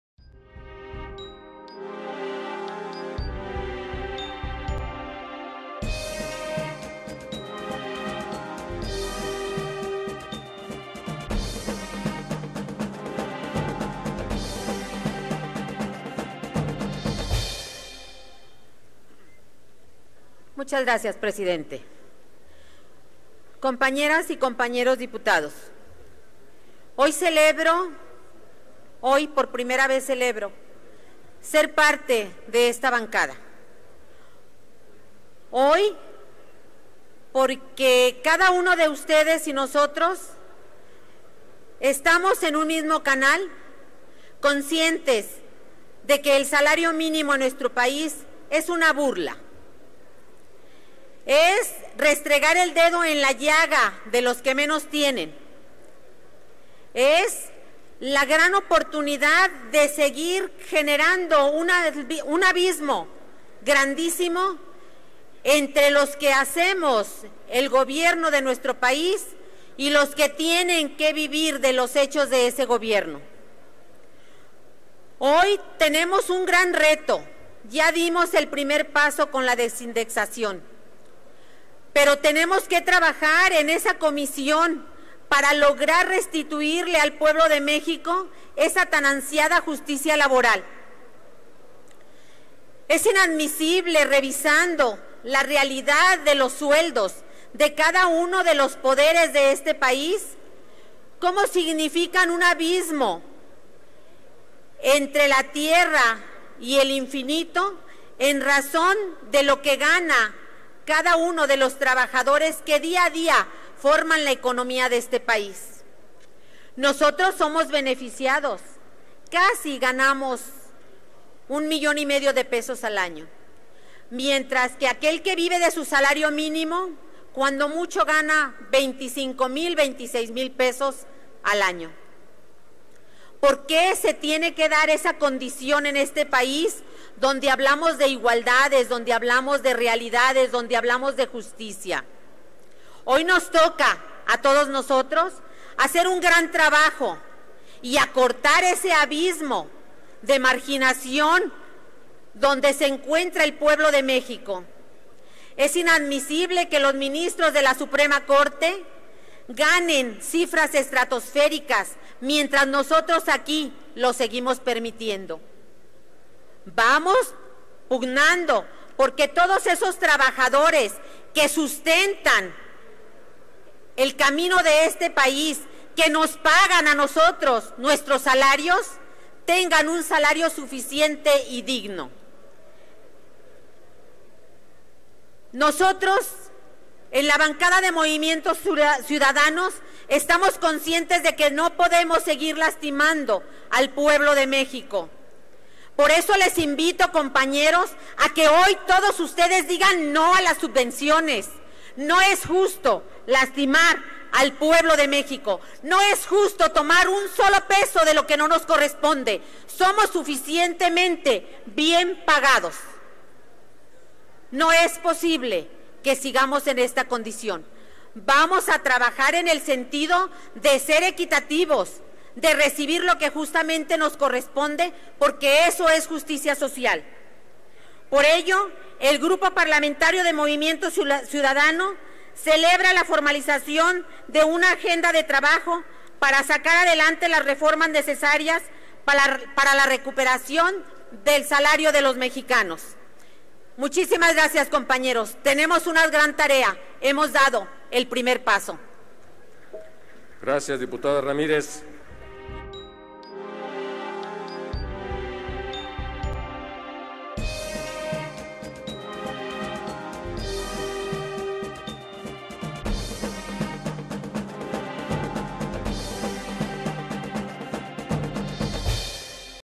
Dip. Rosa Ramírez (MC) - Análisis Iniciativas y Minutas de Salario Mínimo
La Dip. Rosa Alba Ramírez Nachis (MC) indica que la desindexación fue el primer paso para restituir al pueblo de México la capacidad adquisitiva perdida, pero aún queda un largo camino para hacerlo efectivo. Refiere las disparidades en el ingreso entre la clase política y la población. Celebra el establecimiento de la agenda de trabajo para ajustar el salario mínimo.